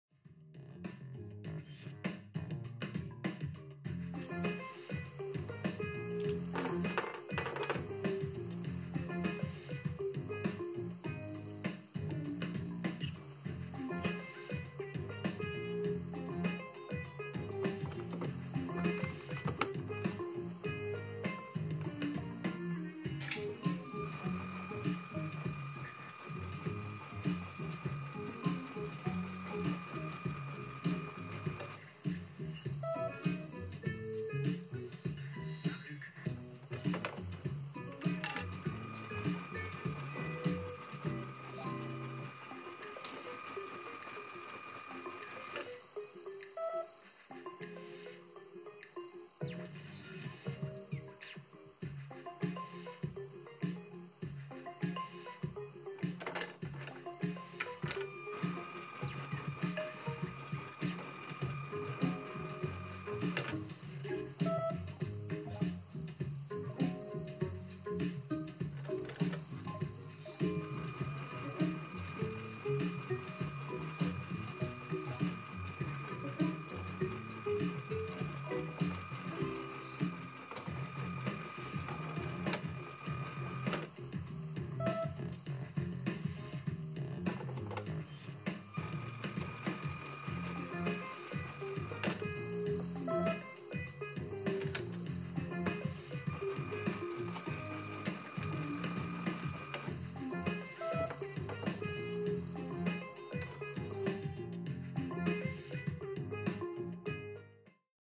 LEGO BOOST Supermarket Cash Register
You can go shopping for LEGO bricks with this LEGO BOOST Cash Register model.

The color and size of each item corresponds to a price tag. On the smart device screen you can read the total of your cart.